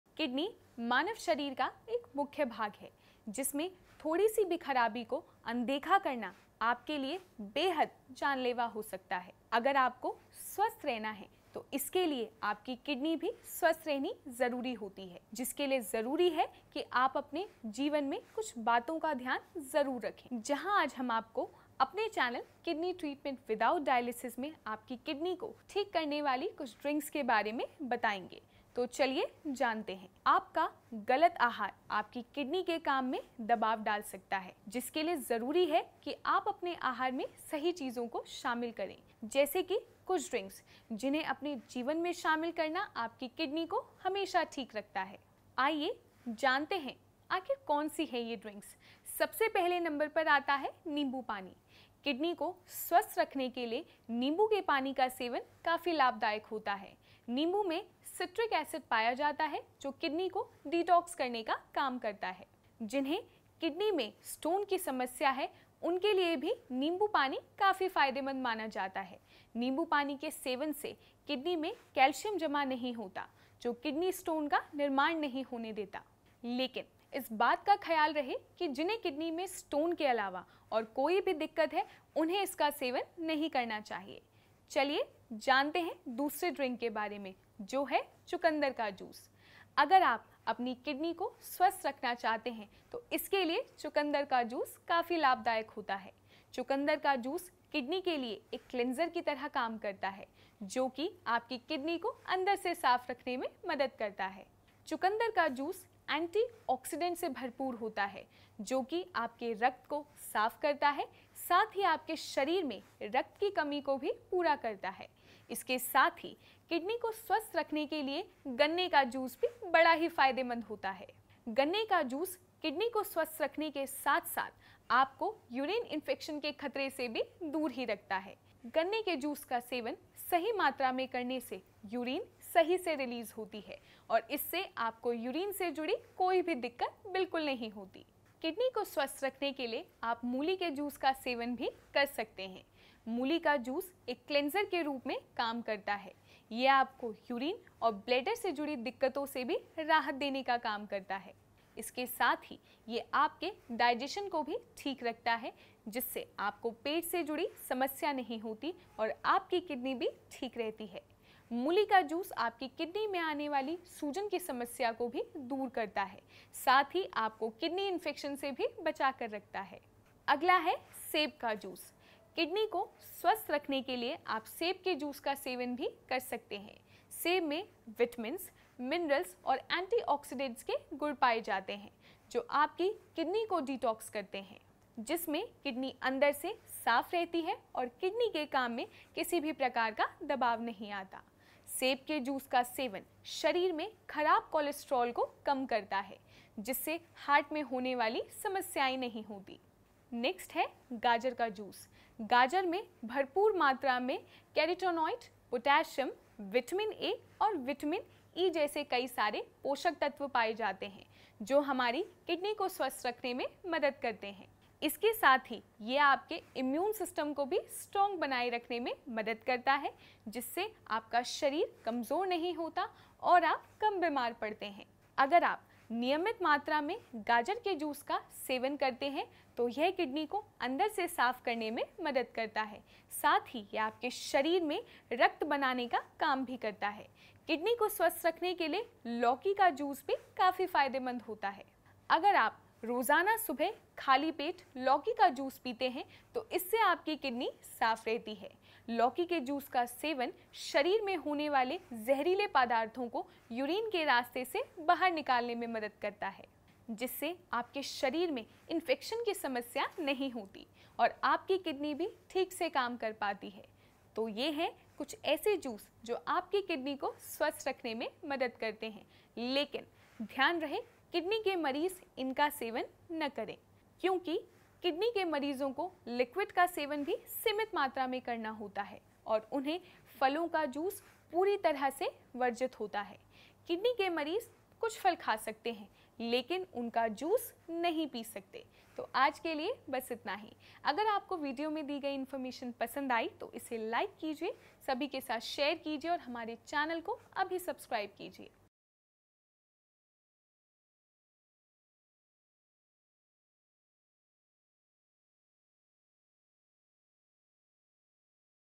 Best Drinks for kidney health | kidney rogi kya pee saktey hai | diet for kidney patients in hindi
In this video, we will share some best drinks to keep your kidneys healthy and strong,
water intake is very important for your kidney health but for kidney disease patients it is very crucial to know how much water intake they should take and in which form.